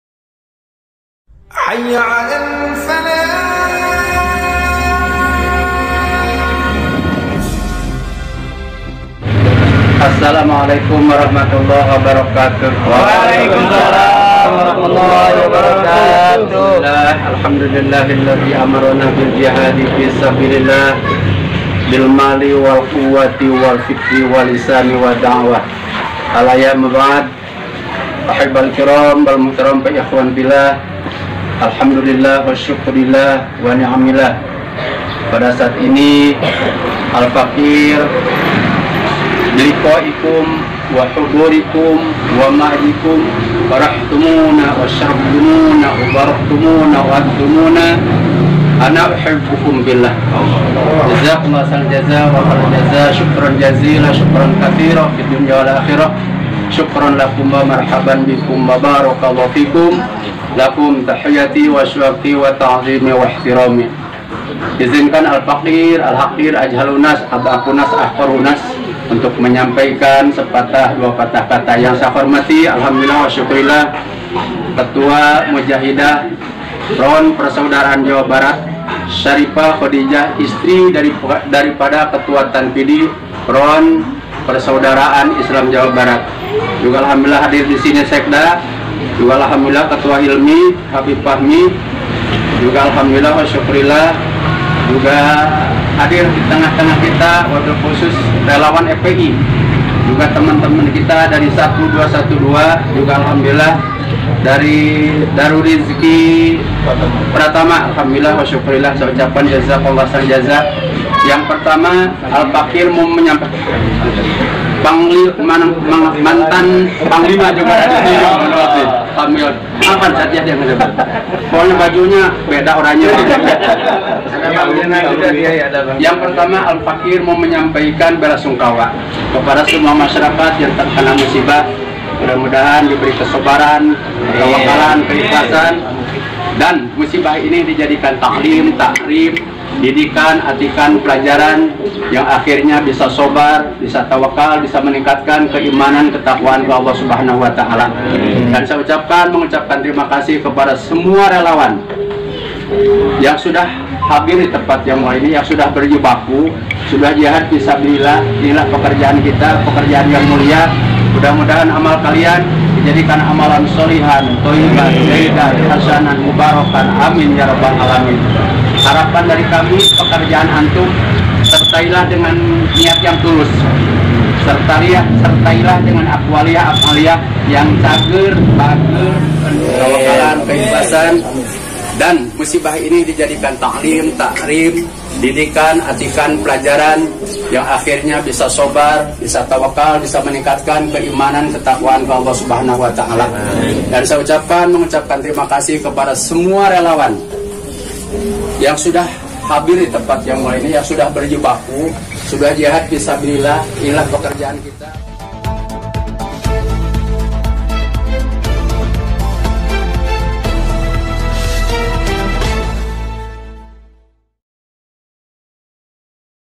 KH MA'SUM HASAN BERIKAN BANTUAN UNTUK KORBAN BENCANA DAN TINJAU POSKO HILMI FPI DI LEUWILIANG BOGOR
KH Ma'sum Hasan Serahkan Bantuan dan Tinjau Posko Banjir HILMI FPI dan PA 212 di Purasari Leuwiliang

Hingga saat ini para Relawan Front Persaudaraan Islam (FPI) yang terdiri dari HILMI dan PRI bersama Persatuan Alumni 212 (PA 212), warga dan elemen lainnya bahu membahu dalam mencari, menemukan dan mengevakuasi jenazah para korban bencana alam banjir bandang dan longsor di Leuwiliang dan Pamijahan, Kabupaten Bogor, Jawa Barat.

Tak hanya itu, para pejuang kemanusiaan ini juga membersihkan lumpur sisa banjir di rumah-rumah warga dan di jalan-jalan, serta berbagai kegiatan lainnya di lokasi banjir bandang dan tanah longsor.

Hari ini, Kamis (7/7/2022) KH Ma'sum Achmad Hasan menyerahkan bantuan berupa uang tunai sebesar hampir 3 juta Rupiah, serta paket sembako, mushaf Al-Qur’an dan peralatan sholat untuk para korban bencana melalui Posko Banjir HILMI FPI dan PA 212 Purasari Leuwiliang Bogor.

Sosok yang juga merupakan  Penasehat Umum Pondok Pesantren Miftahul Huda Utsmaniyyah Cikole Kecamatan Cihaurbeuti Kabupaten Ciamis itu menyempatkan perasaan belasungkawa kepada masyarakat yang sedang menerima musibah dan mendapat ujian dari Allah SWT ini.

Beliau kemudian memberikan dorongan semangat kepada para Relawan FPI (HILMI dan PRI), PA 212 , Darul Rizky Pratama dan lainnya yang telah berjibaku di lokasi bencana dan membantu masyarakat.

Kehadiran Kyai Ma'sum disambut gembira oleh para Relawan.

"Kehadiran dan Nasehat nya mjadi semangat relawan dilokasi bencana leuwiliang Bogor 7 Juli 2022.", ujar seorang Relawan FPI.

Sumber : Faktakini.info
Leuwiliang - Bogor 7 Juli 2022